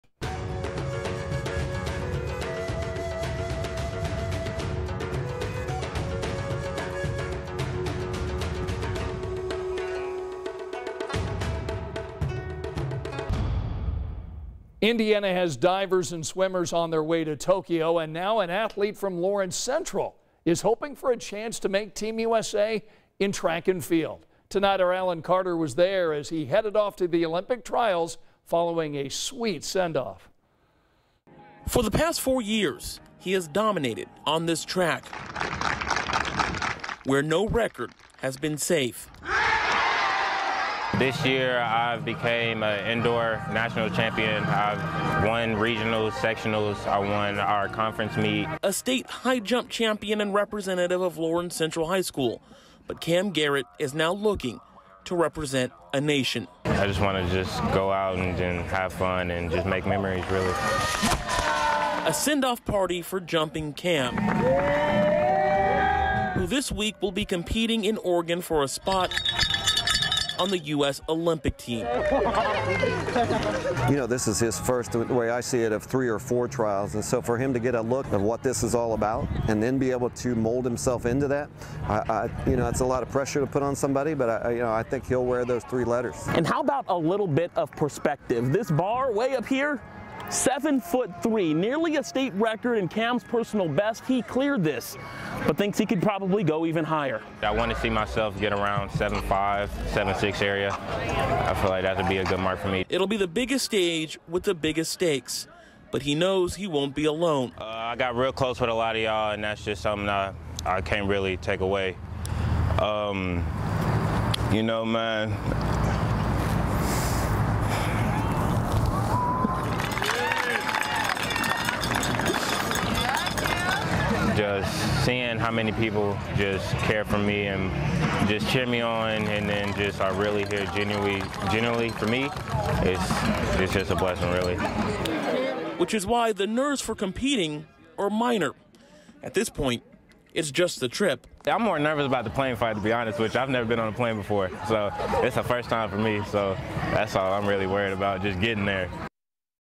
Lawrence Central's Kam Garrett heads to Olympic Trials
A Lawrence Central graduate and state high jump champion will have a chance to punch his ticket to Tokyo this week at the U.S. Olympic Trials.

Kam Garrett heads out Tuesday to compete at the trials in Eugene, Ore.

“I just want to go out and have fun and just make memories, really,” Garrett said.

But Garrett’s coaches believe the 18-year-old has a real shot at earning a spot on the team. Earlier this year he cleared 7'3" during a competition, just one inch shy of a state record.

BY: Allen Carter